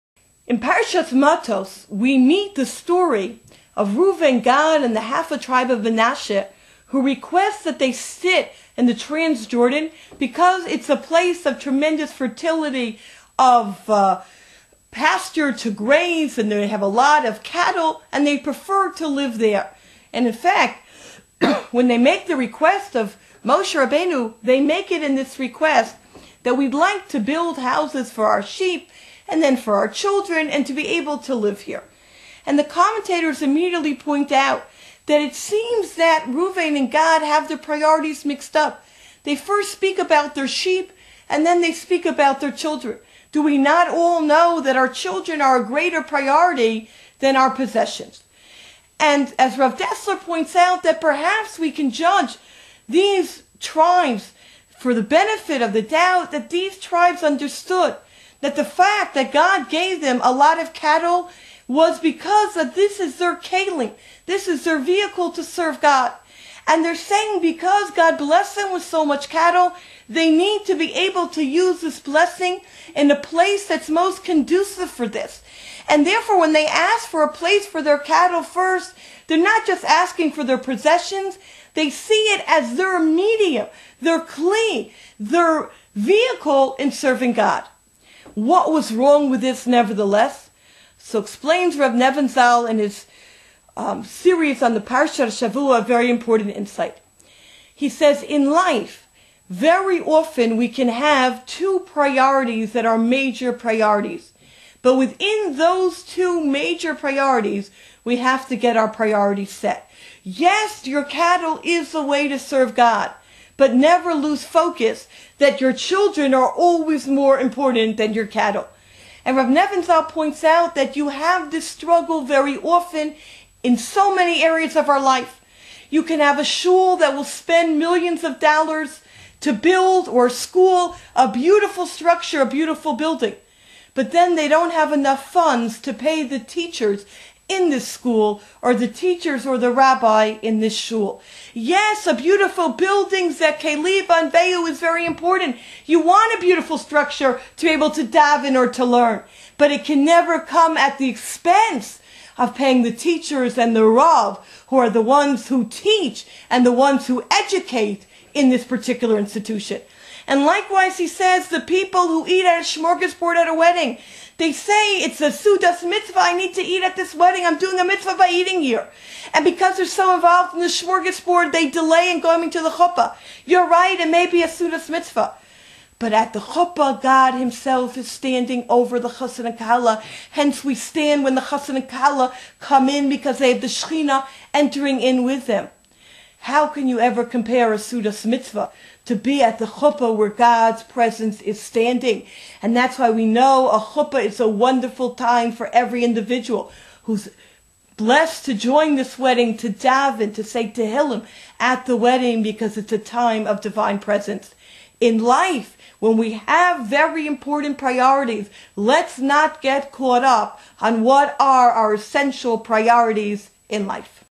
Jewish Torah Insights: Priorities - Parshat Matot-Masei
By Mrs. Shira Smiles
In this week Torah Portion, Parshat Matot-Masei, we learn about how the tribes of Reuven and Gad wanted to settle by the Jordan river for the sake of their. G-d blessed them with lots of cattle and it was therefore through taking care of their cattle that they served G-d. But, we must not lose focus that are children take precedence over our cattle. We must make children and their education our priority today.